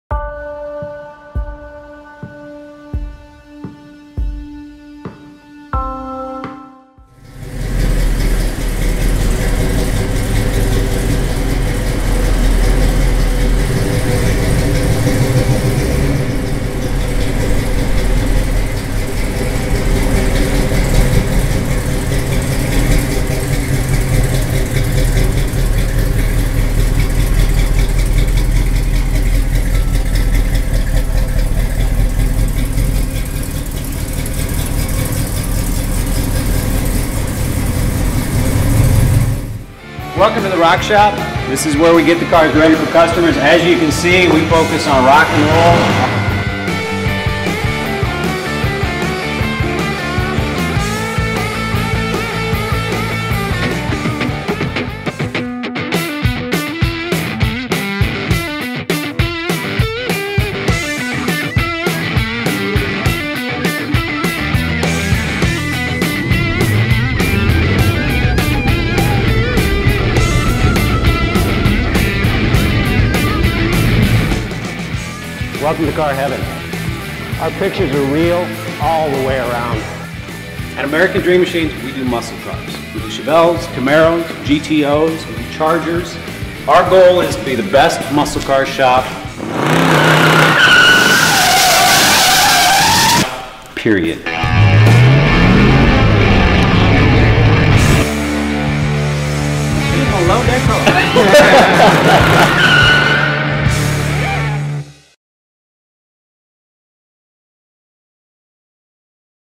1969 Chevrolet Camaro 396
Electric Blue With White Stripes 1969 Chevrolet Camaro Convertible Running A Strong 396 Big Block, 700R4 Automatic Overdrive Transmission And 12 Bolt Posi Rear. Rolling On Polished Aluminum American Racing Wheels And Performance Tires.